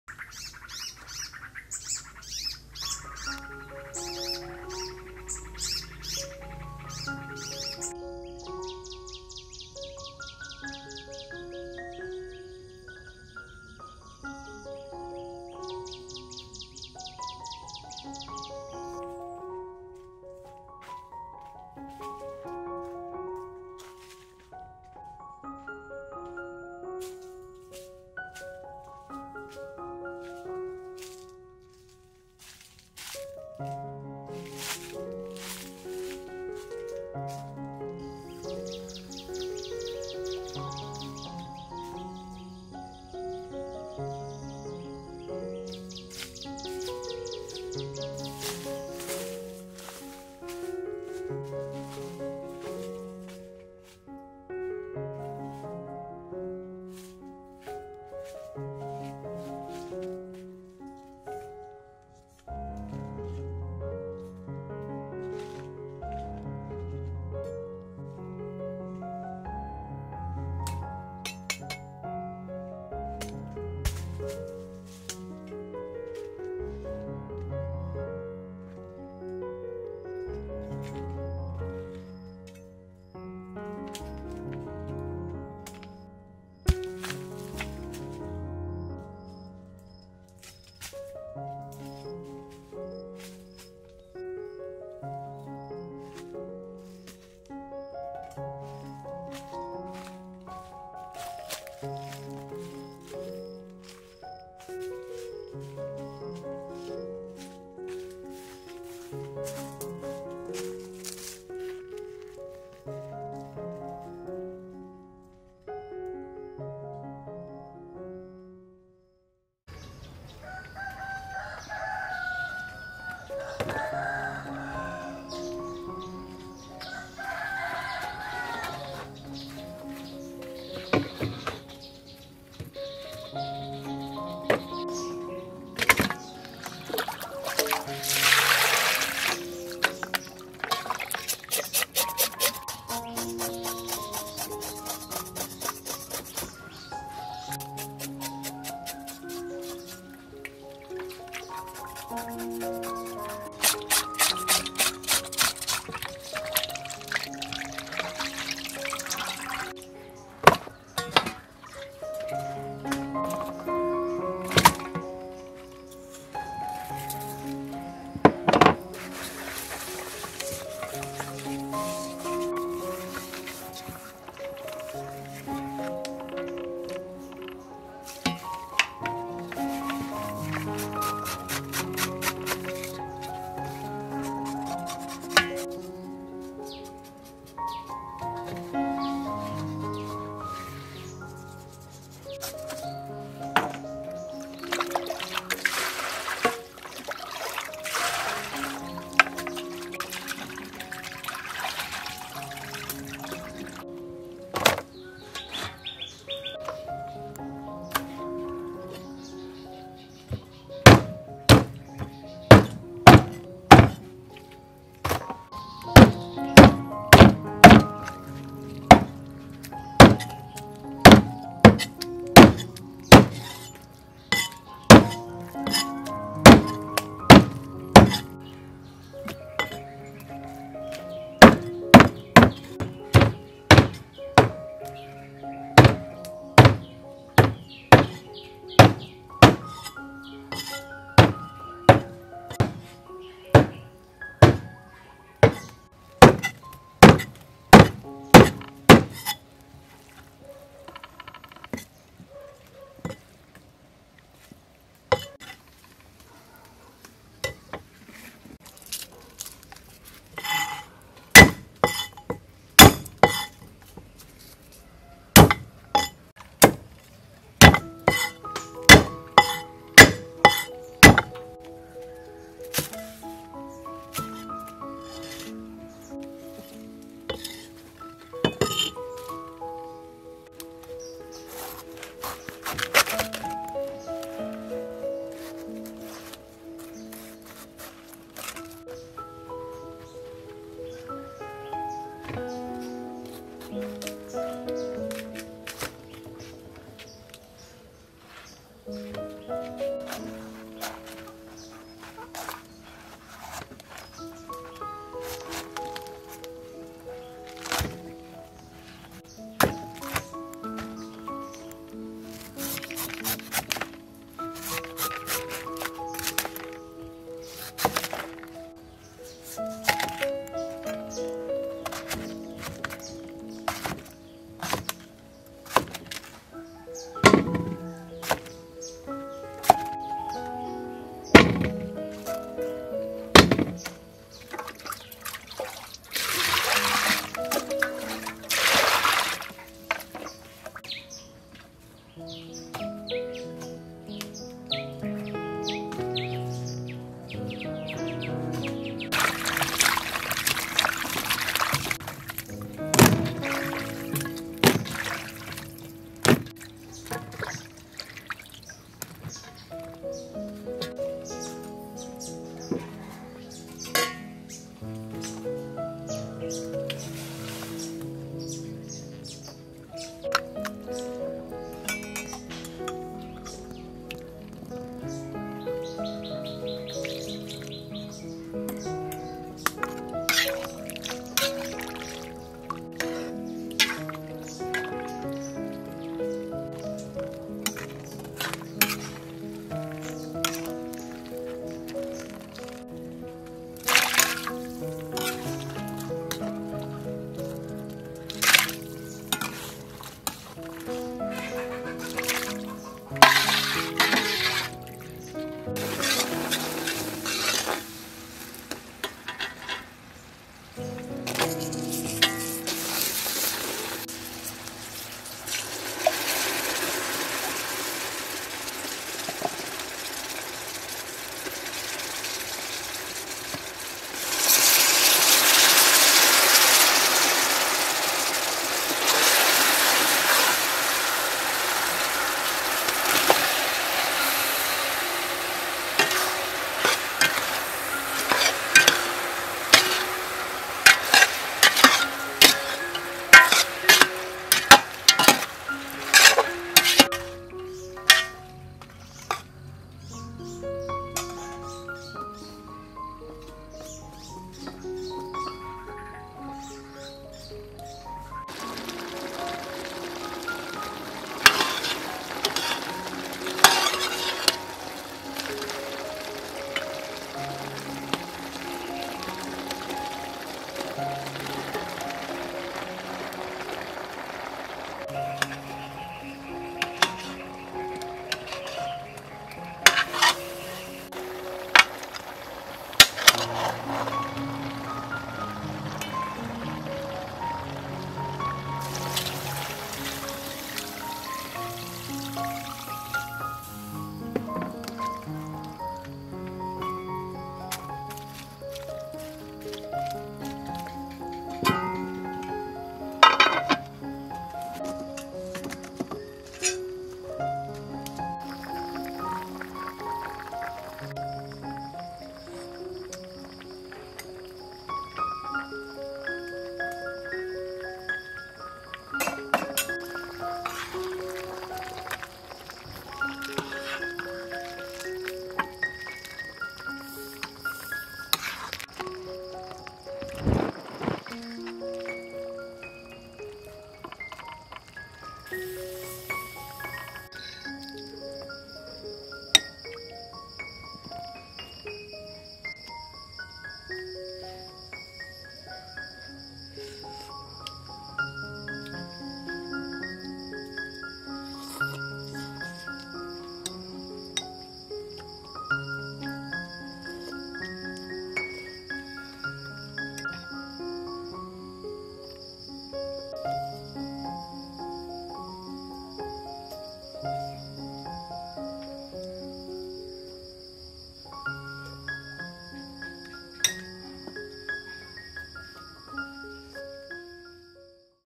Jackfruit in grandmother's backyard is good for cooking / Jack fruit recipe / Cooking with Sreypov
Hello friends. Today I go to find jackfruit for my recipe. I harvest it from my grandmother's backyard. It is good for cooking.
Have you ever used jackfruit for your recipe?
I hope you like my video. Let's enjoy and relax together. Thank you.